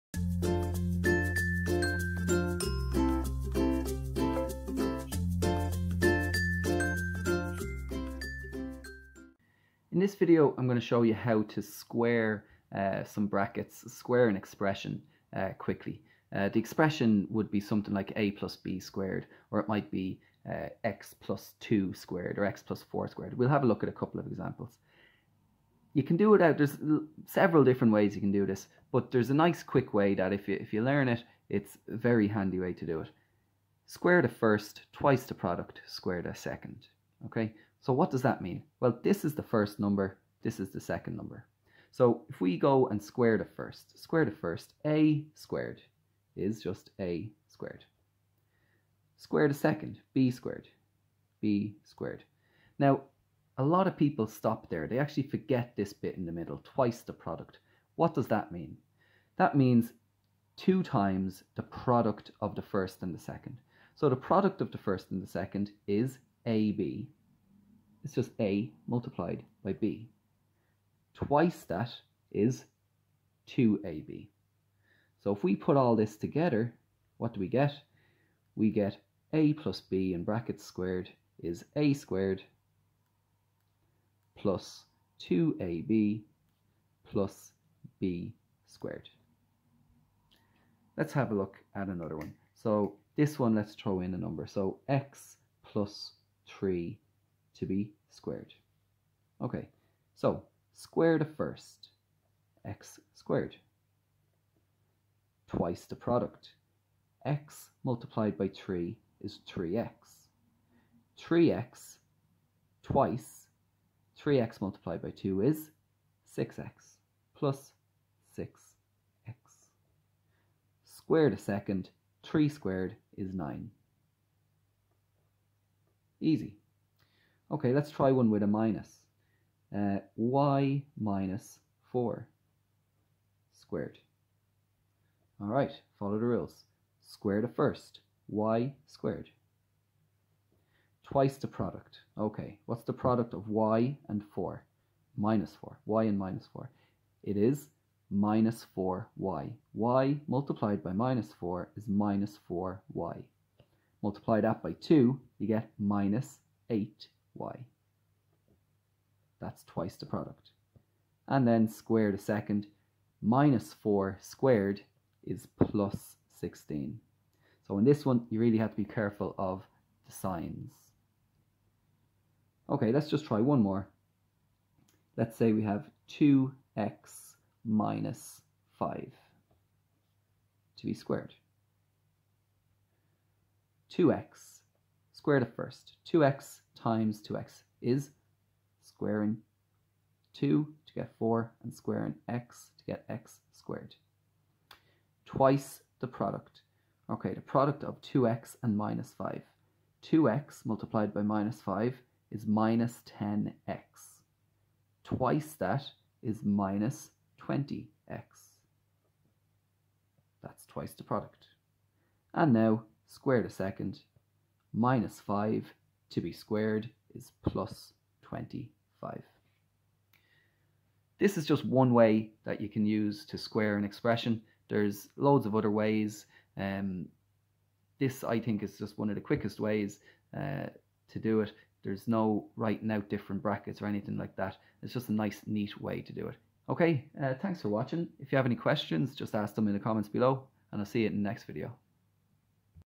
How to Square an Algebraic Expression
In this video I show you the easiest way to square an algebraic expression, also known as squaring the brackets or squaring the parentheses. There are many ways to do this but in my opinion this is the easiest for anybody who is good at maths. If you find maths difficult then there are other ways that may be easier for you.